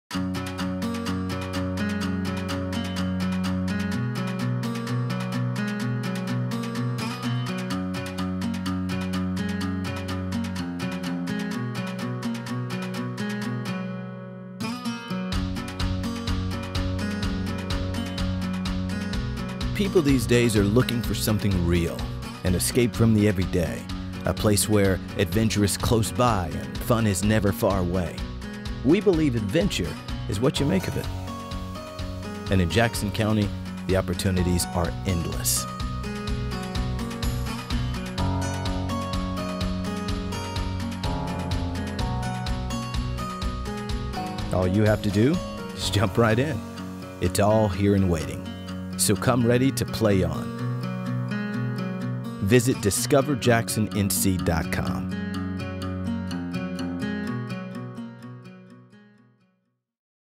Jackson County Tourism - Play On
You're looking for a place that's real and true and full of great things to do. And Jackson County, North Carolina, is the perfect spot. No matter what your idea of adventure is. All you have to do is jump right in. It’s all here, so come ready to Play On.